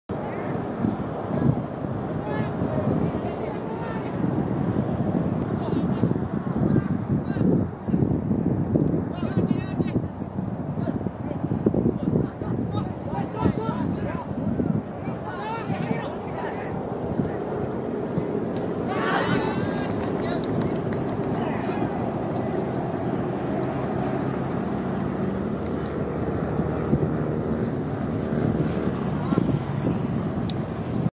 171008 東海ルーキーリーグ15節 藤枝東vs常葉橘 後半10分 藤枝東 8柳谷冬羽 先制決勝ゴール
アシスト：19澤口裕斗